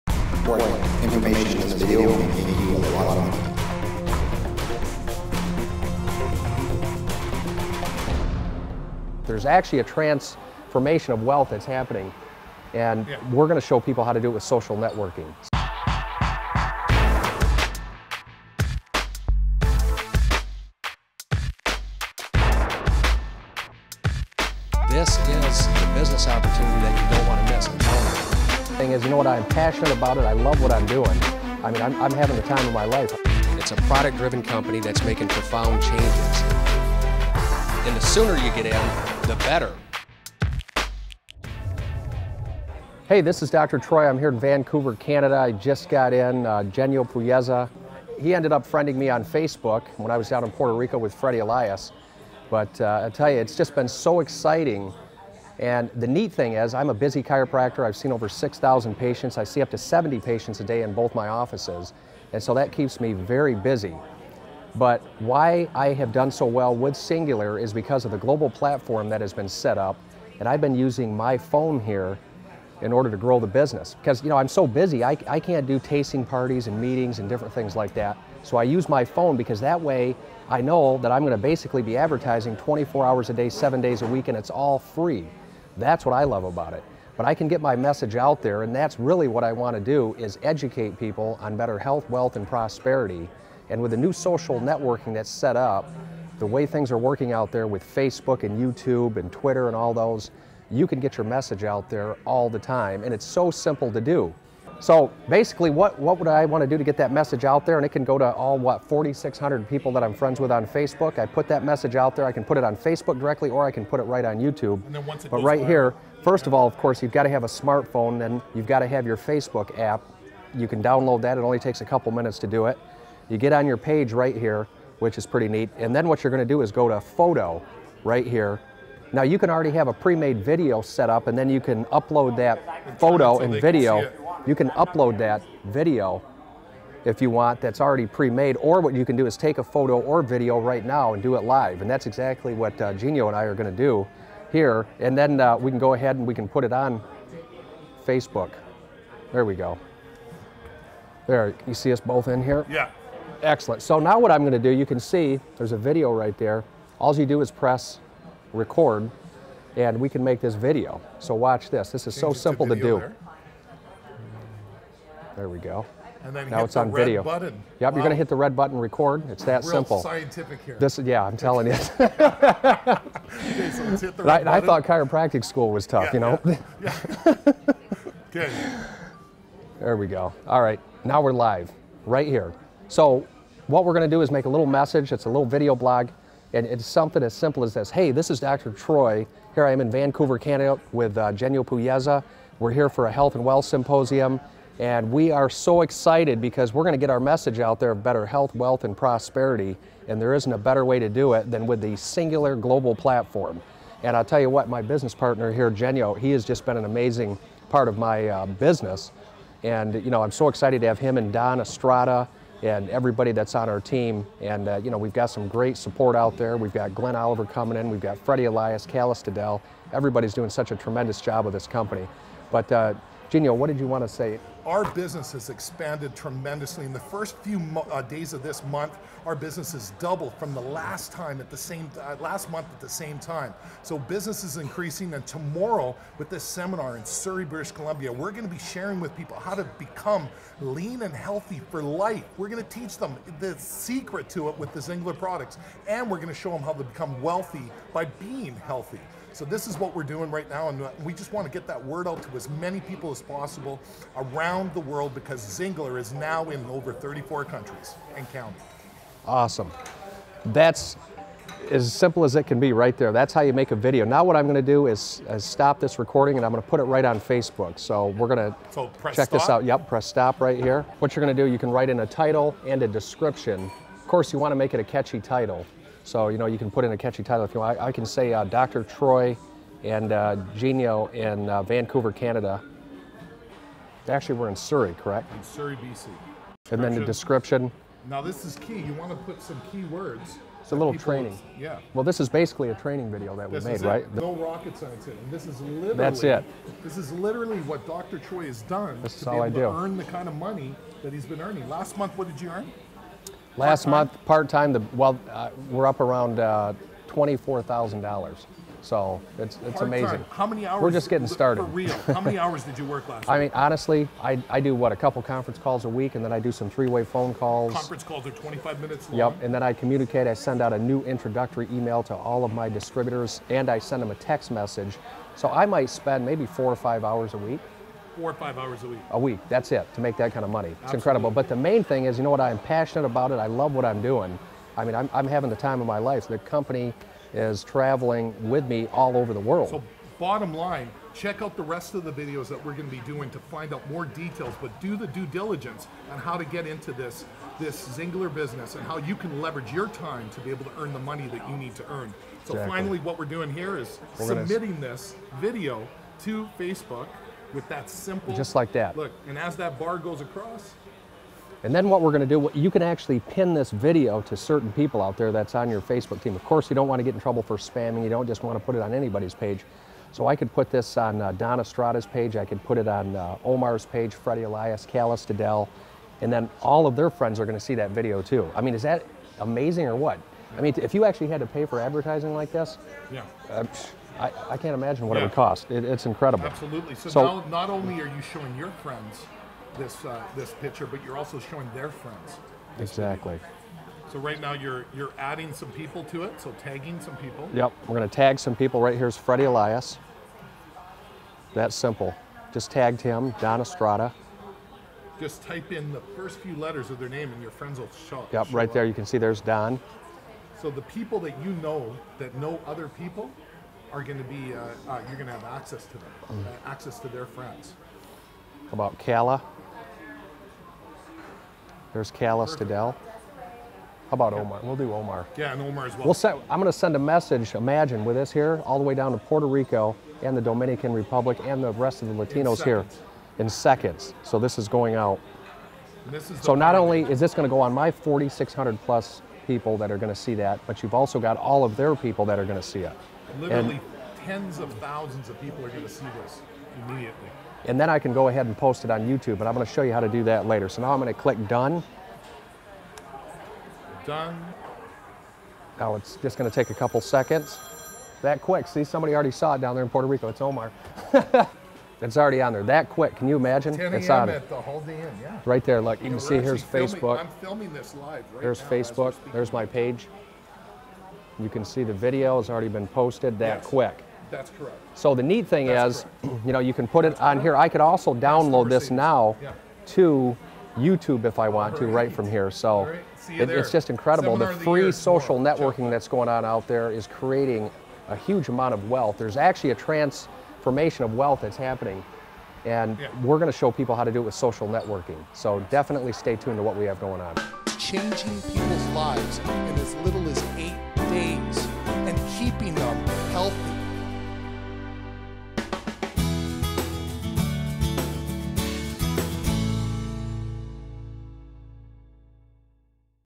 Uploading Video to Facebook-XTeam Health and Wealth Training.mov
Dr. Troy McHenry sat down when he arrived at the XTeam Global Health and Wealth Seminar in Surrey, B.C. Canada to explain how he uses social media to get the message out across the world.  In this training video Dr. Troy uses his smart phone to capture a quick video and uploads it within seconds to his facebook account.  See how he is impacting thousands of people's lives. Filmed and Edited by Jeremy Deichen.